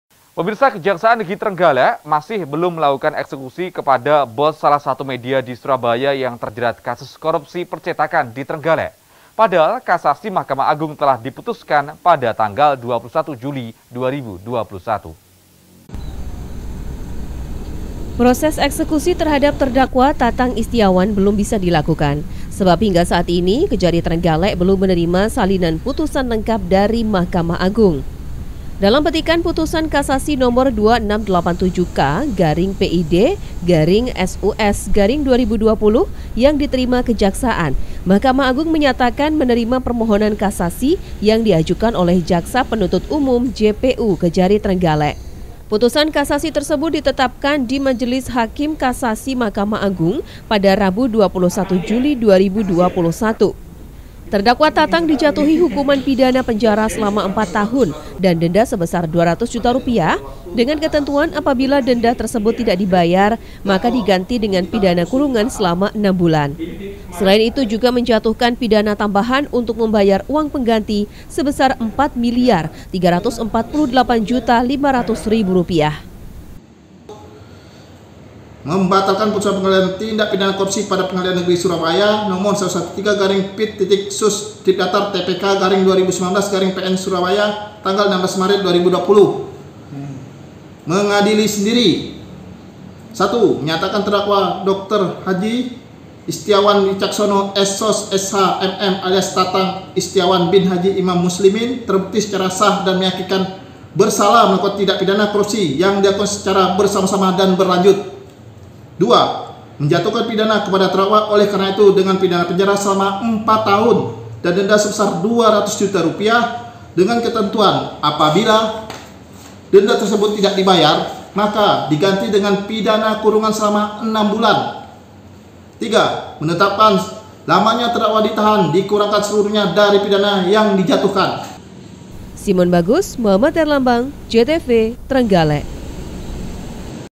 TRENGGALEK - KEJARI Belum Eksekusi Terpidana Korupsi Bos Media Di Surabaya
Kejaksaan negeri trenggalek masih belum melakukan eksekusi kepada bos salah satu media di surabaya yang terjerat kasus korupsi percetakan di trenggalek. Padahal kasasi mahkamah agung telah diputuskan pada 21 juli 2021.